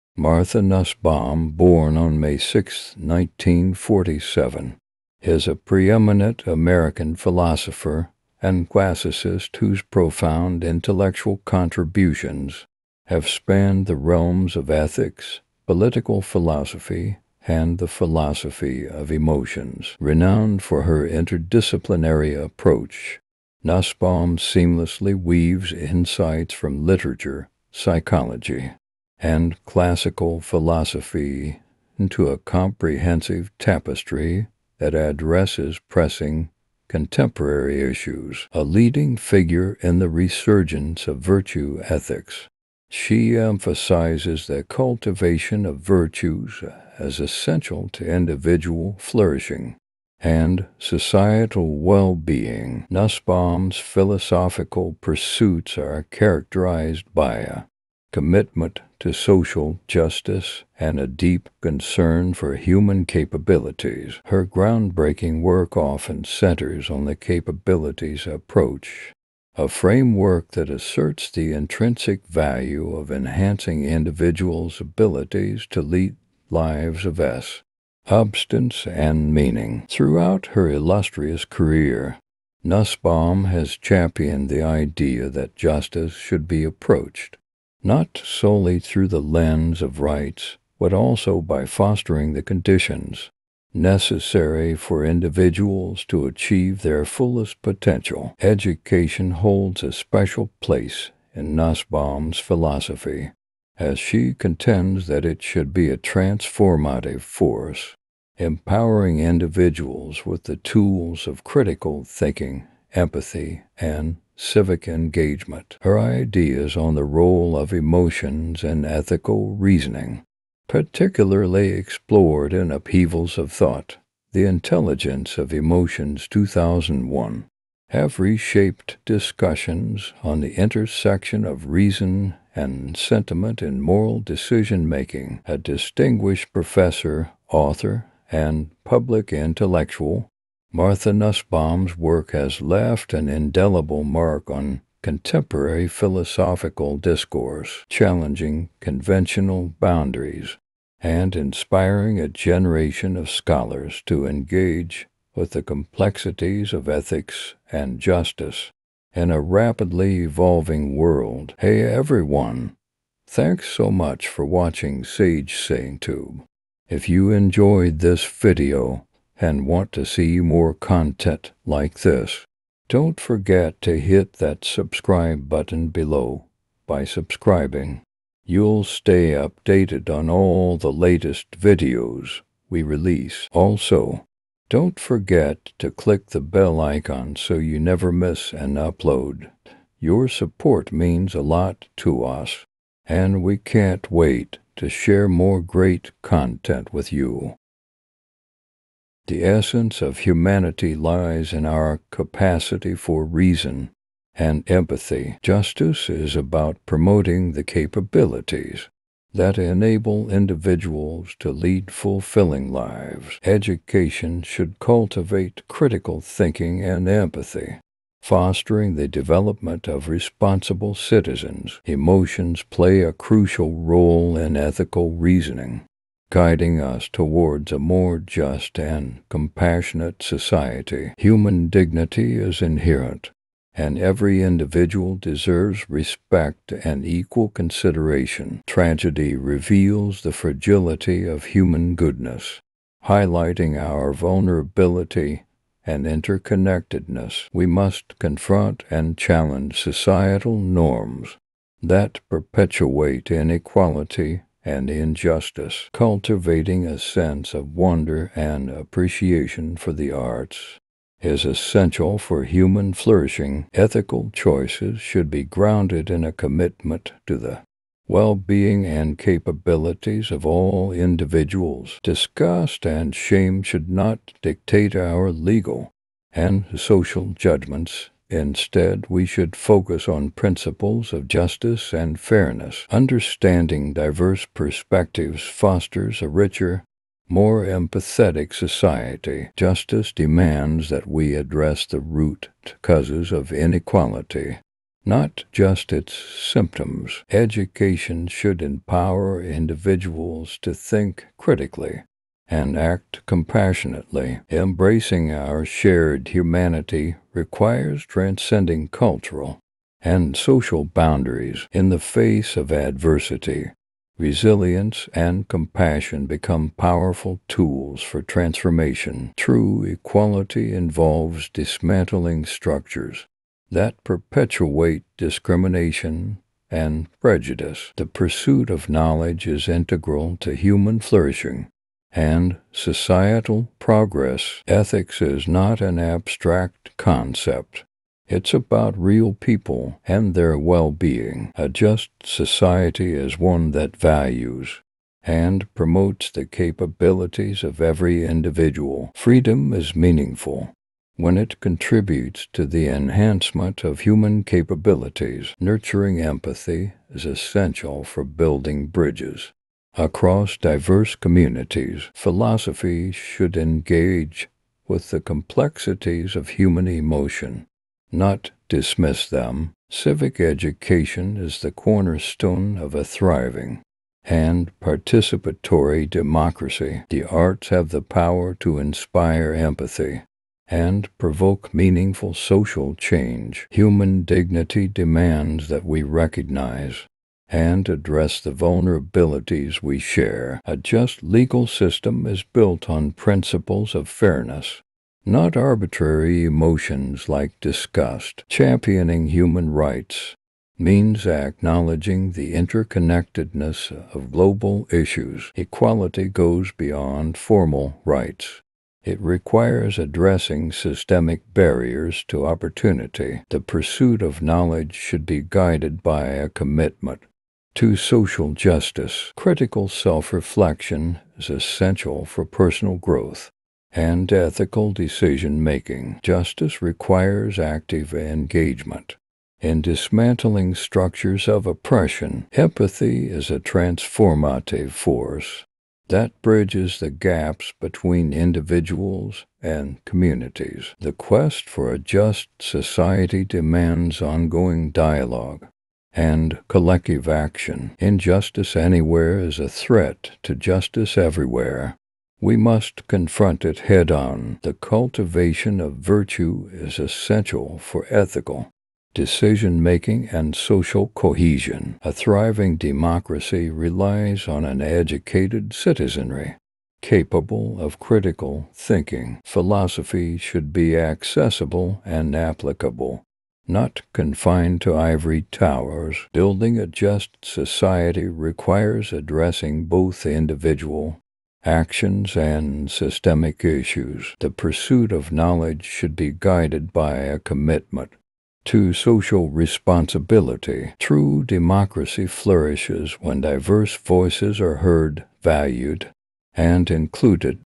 Martha Nussbaum quotes
"Embark on a philosophical journey with SageSayingTube as we delve into Martha Nussbaum's profound insights on justice, human flourishing, and the transformative power of education. Join the discourse on virtues and capabilities that shape a more just and empathetic world. 🌍✨